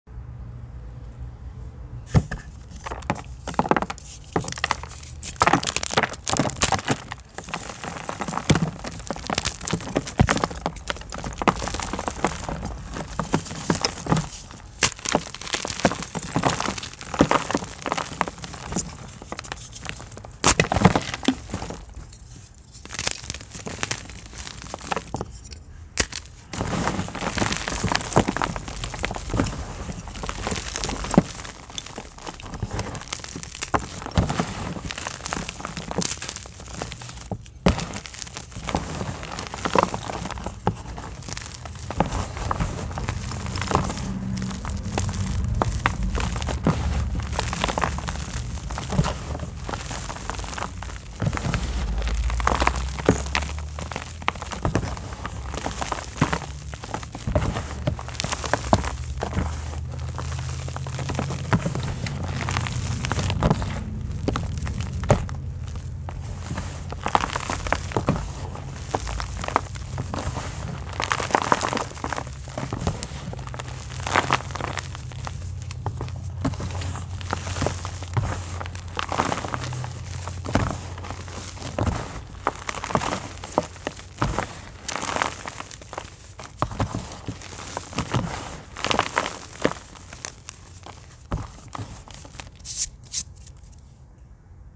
3 Picsil gymchalk Crush topped with paste asmr/oddly satisfying
Hope u enjoy This 3 block Crush topped with cornstarch paste 🥰

Crush is up to 0.45